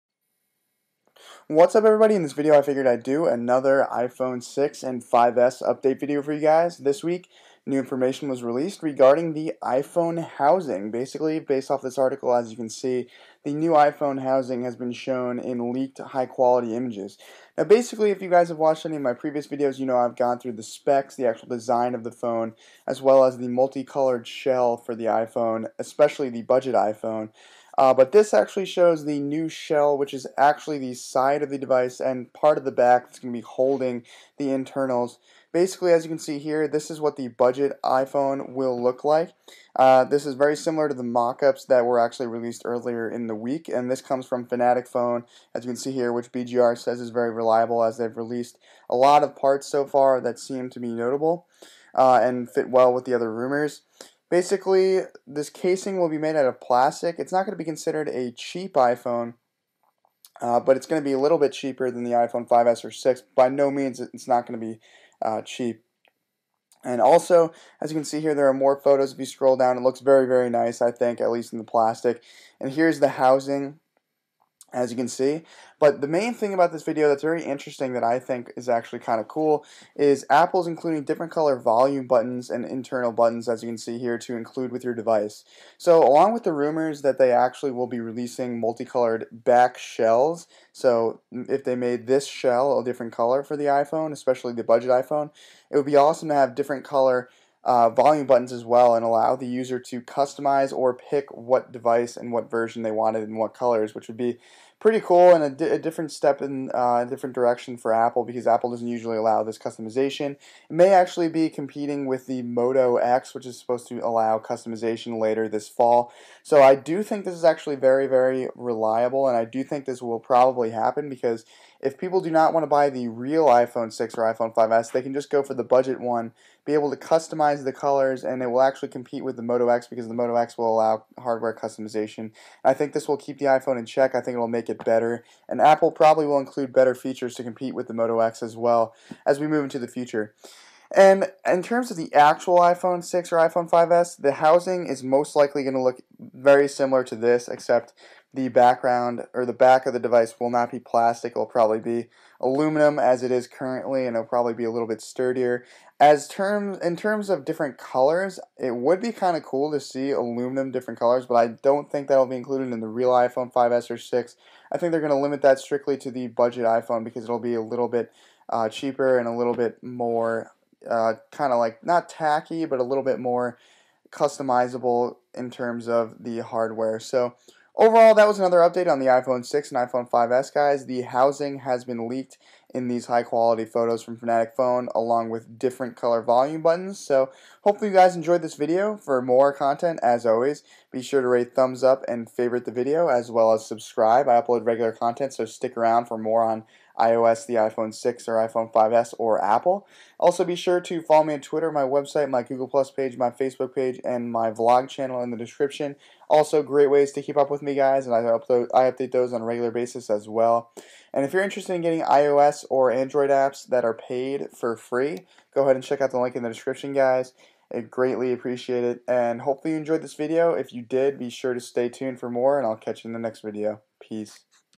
iPhone 6 / 5S Housing Leaked: High Quality Photos (BGR)
According to BGR and other websites, the iPhone 6 or iPhone 5S Housing has been leaked in high quality photos! The housing is supposed to look very nice, be made out of aluminum and plastic for two different phones, and maybe even have different colors or different color volume buttons! The iPhone 6 or 5S is supposed to have many different features itself as well. Thanks!

Sign up on a Mobile Device to Earn Rewards by trying free apps on iOS or Android